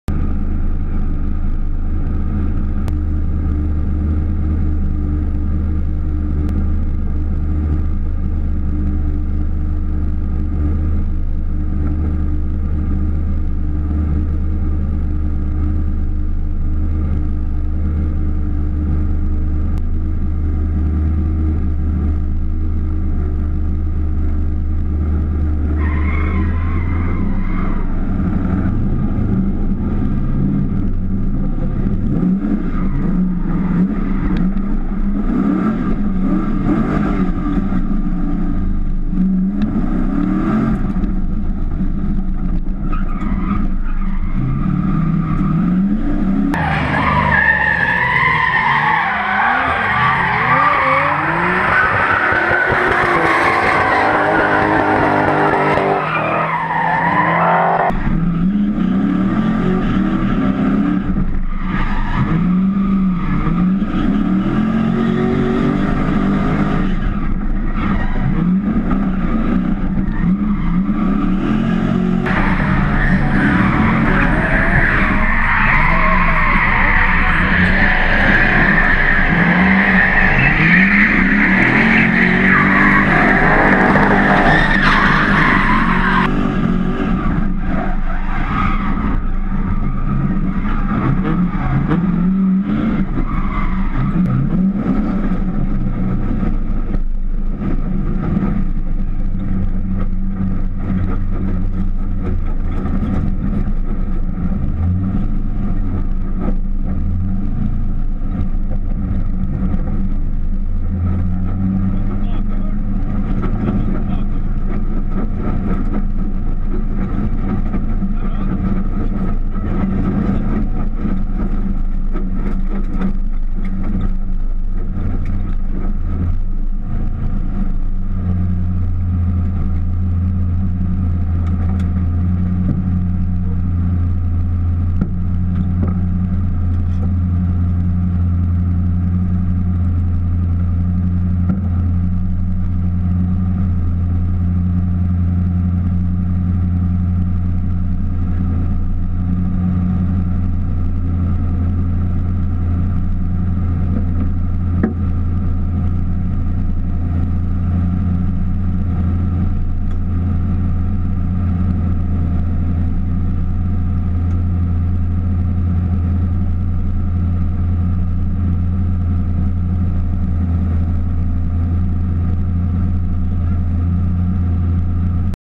Steffen Rudsengen
Morsomt å sitte på med en som kan drifting , eller kjøre kontrolert idiot , Rudsengen fra DTE drifting team kan det